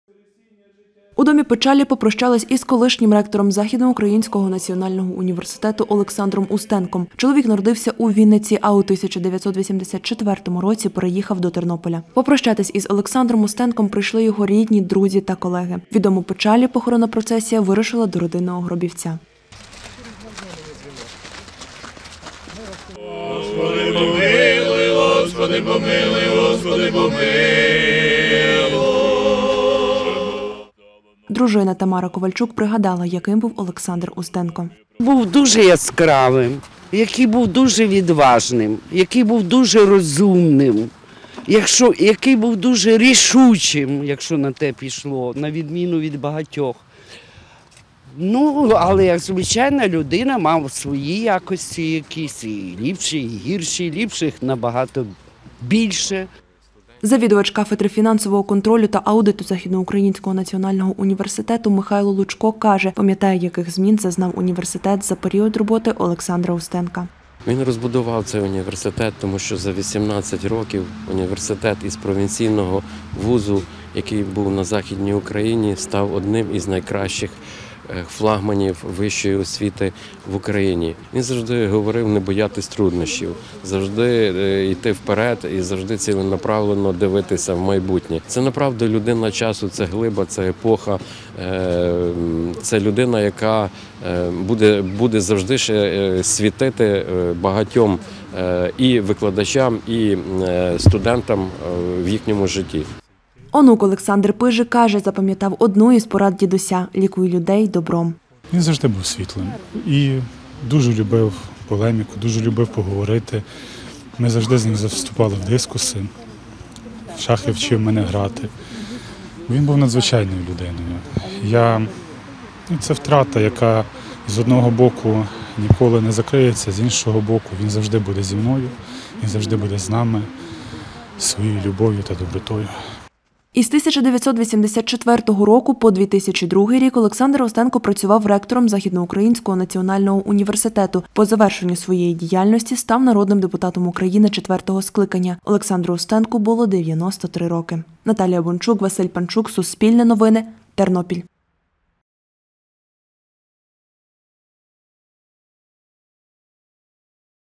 Попрощались із Олександром Устенком
У Тернополі в Домі печалі попрощались із колишнім ректором  Західноукраїнського національного університету та народним депутатом  України четвертого скликання Олександром Устенком. Йому було 93 роки. Вшанувати пам'ять Олександра Устенка прийшли його рідні та колеги.